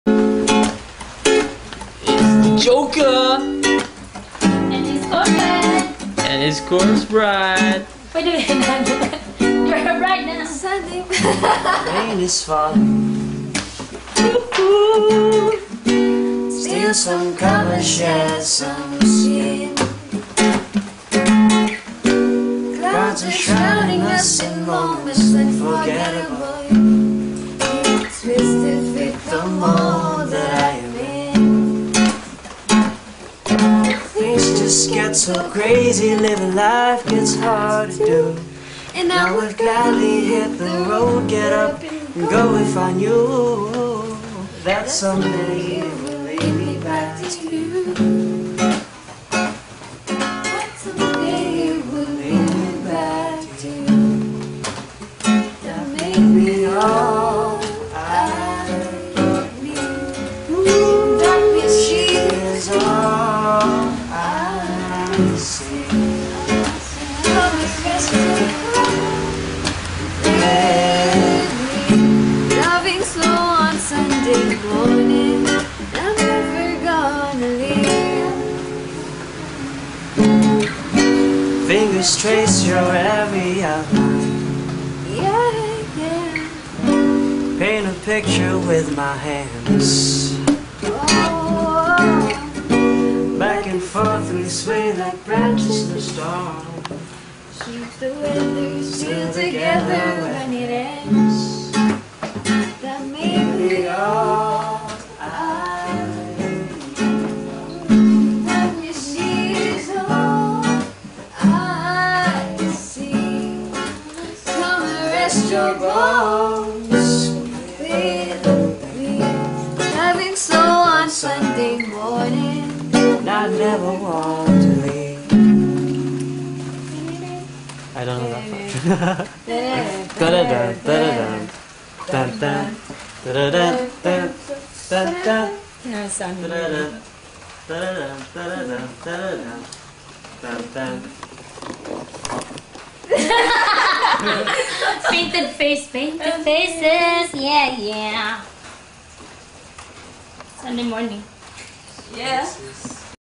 facepaint singers
joker, corpse bride, unfinished avatar, camera person and creator try to sing the night away haha! enjoy!